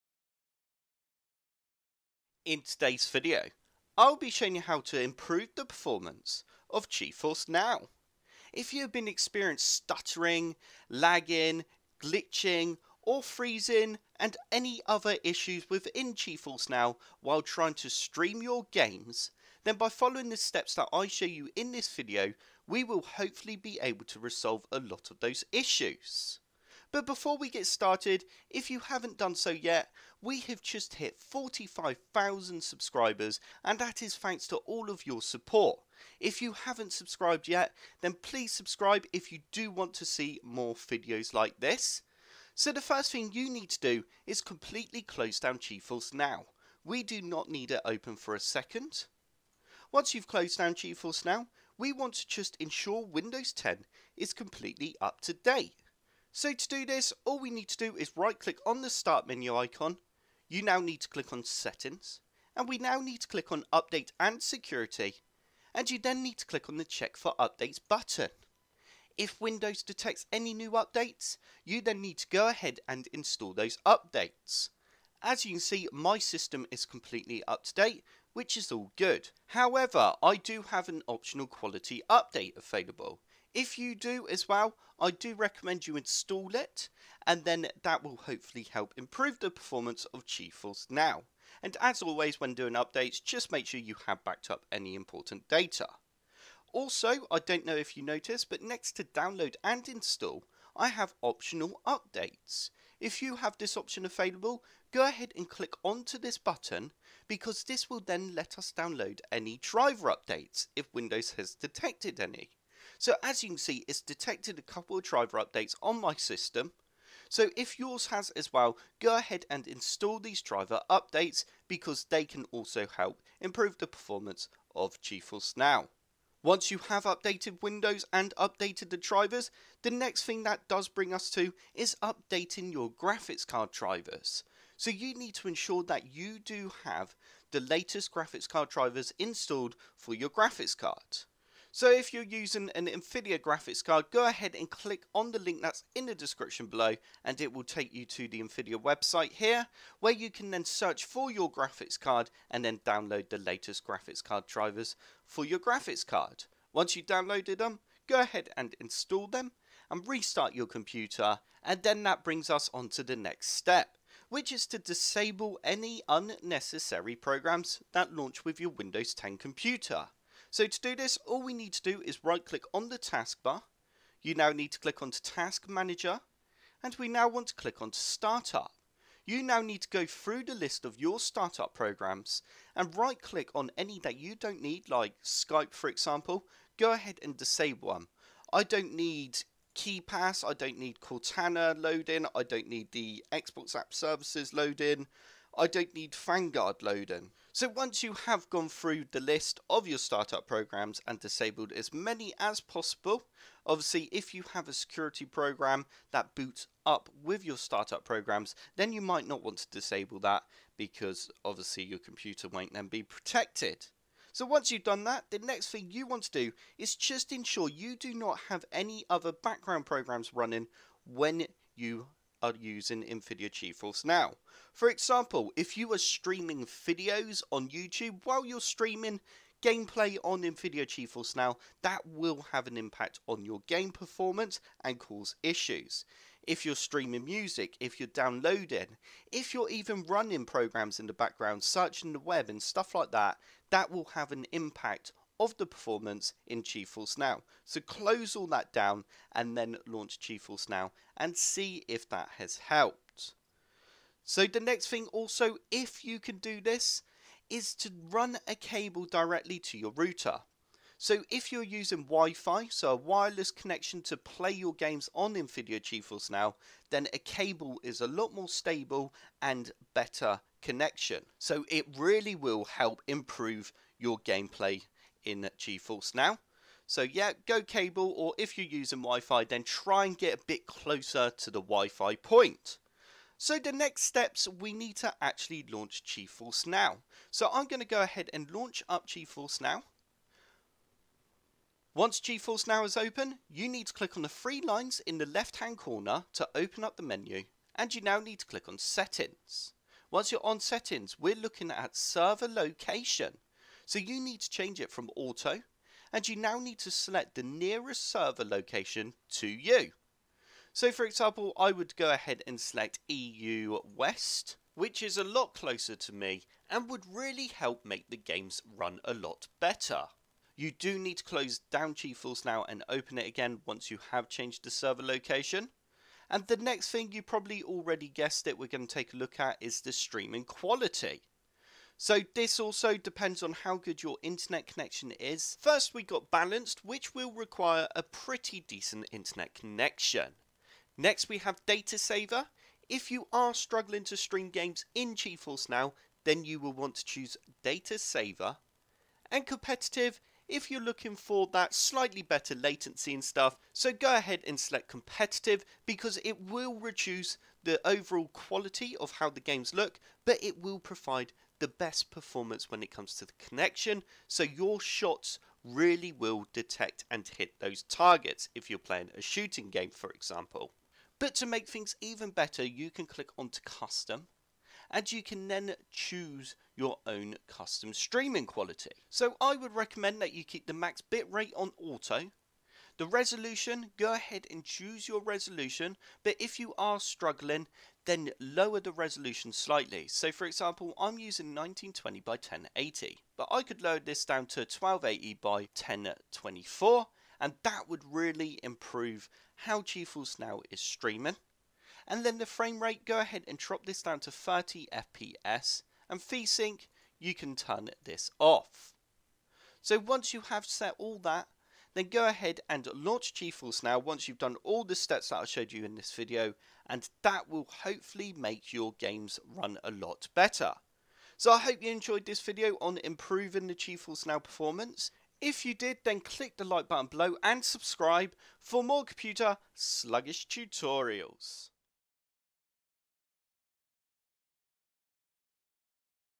Tweak GeForce Now & Improve Performance
Make Nvidia GeForce Now Run Better On Your Computer

In this Windows tutorial, I will be showing you how to improve the performance of GeForce Now on your PC or laptop. If you have been suffering from poor performance within GeForce Now such as the games are lagging, stuttering, or freezing, then by following the steps within this video we will hopefully be able to get Nvidia GeForce Now running a lot smoother and better on your computer so the gameplay will be a lot more enjoyable!